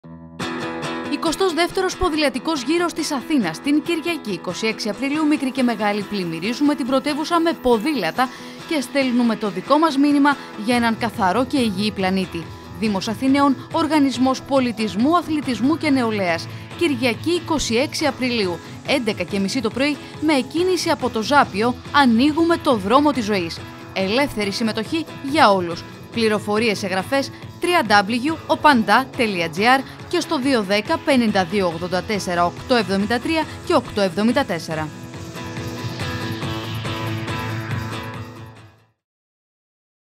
22ος Ποδηλατικός Γύρος της Αθήνας
Την Κυριακή 26 Απριλίου πλημμυρίζουμε την πρωτεύουσα με ποδήλατα και στέλνουμε το  δικό μας μήνυμα για έναν καθαρό και υγιή πλανήτη!